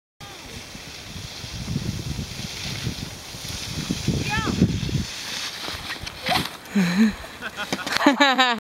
Tingle Boys Snowboarding
Addison and Chandler Snowboarding on Golf Course